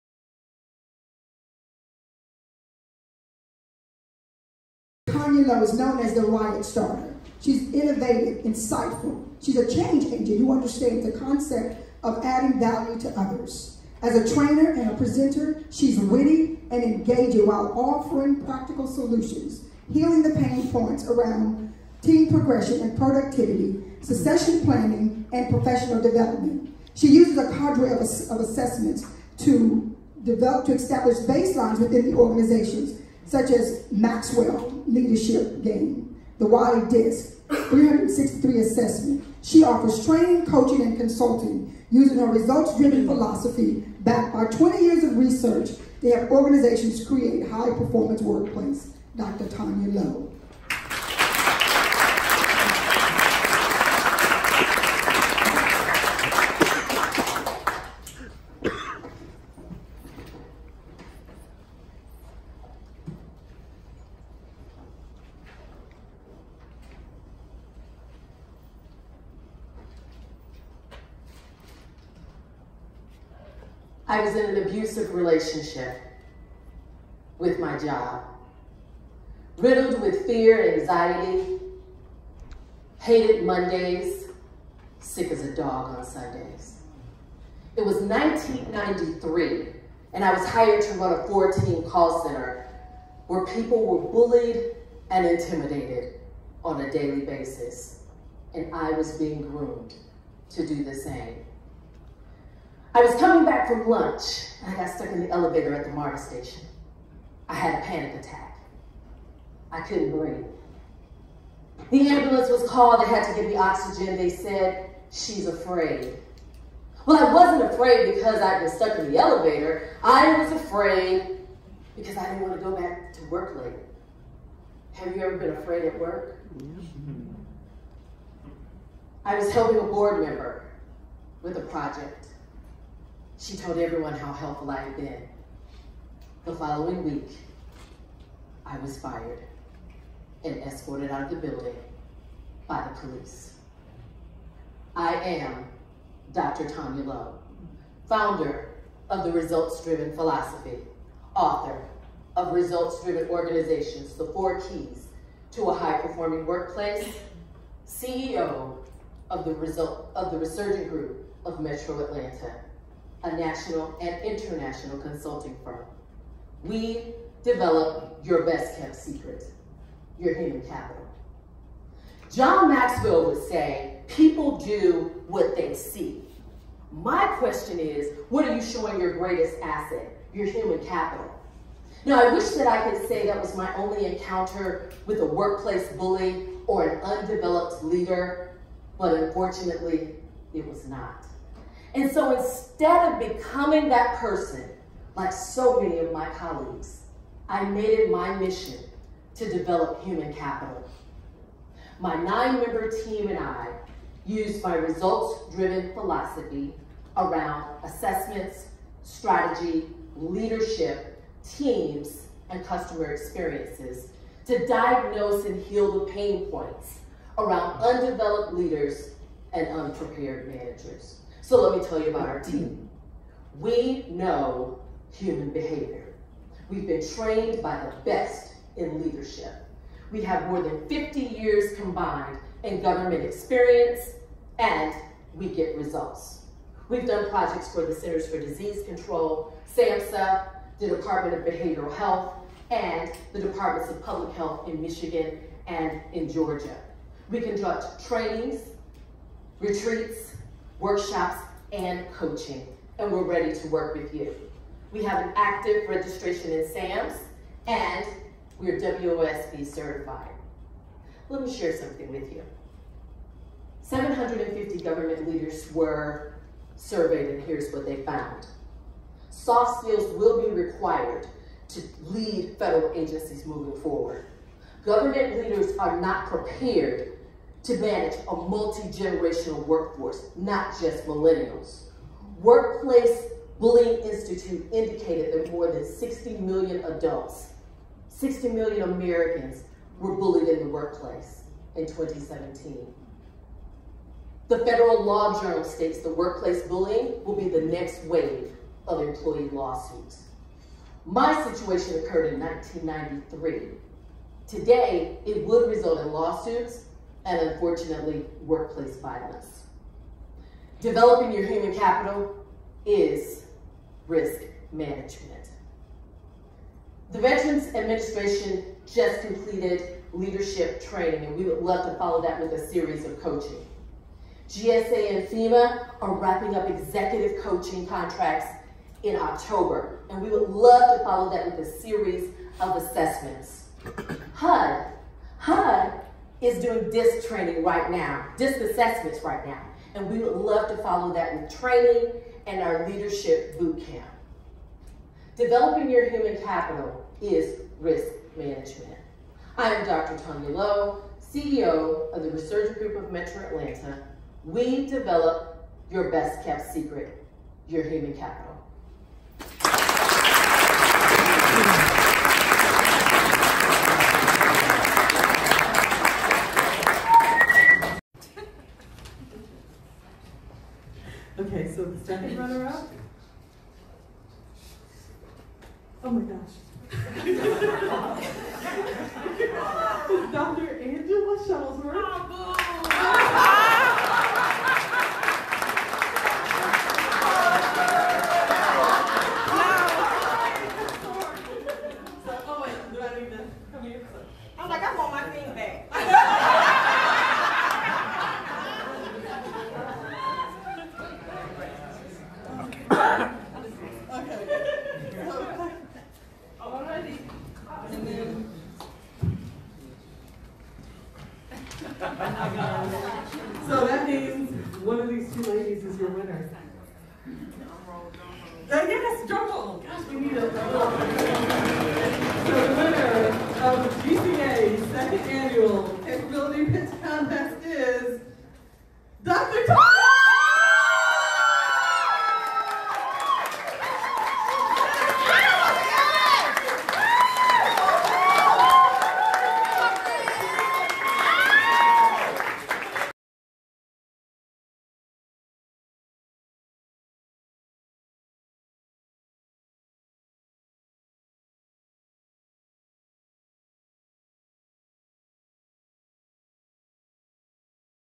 Dr. TAUNYA A. LOW 2019 GCA CAPABILITY PITCH CONTEST WINNER
THIS is my SEASON! I AM dripping in FAVOR! Giving ALL honor and praise to God! I AM THE 2019 Government Contractors Association Capability Pitch Contest WINNER!!!